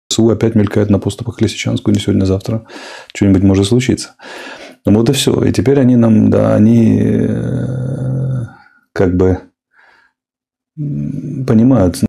Мантра от хуторянина Арестовича: "две-три недели максимум"🙉🙈🙊
Даже отбитые украинские националисты уже поняли всю суть личности по имени Алексей Арестович, добавив его в список врагов государства... А верные зрители всё ещё продолжают смотреть новые эфиры Люси, ожидая скорую перемогу.

ЦИФРОВАЯ САТИРА

@arestovych  и текст и картинка были нарисованы 6 июня 2022 года 🤷‍♀️